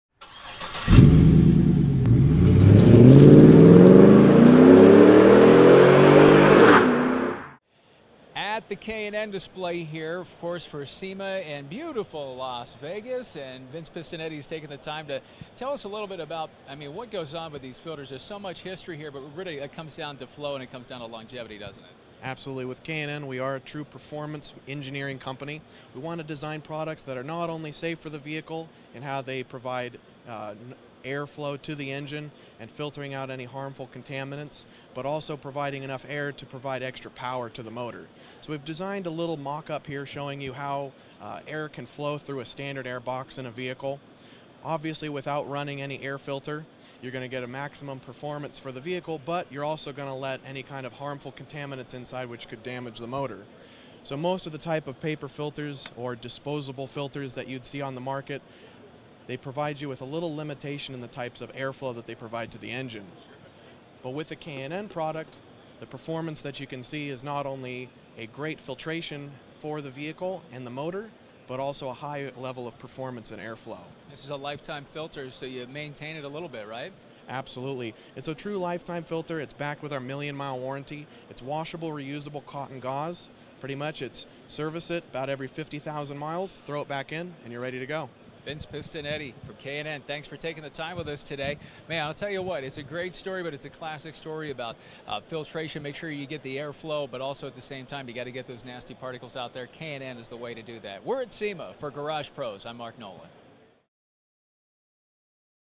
K&N How their air filters work
See K&N's mock up of a working air box like the one in your vehicle.  Pretty cool stuff.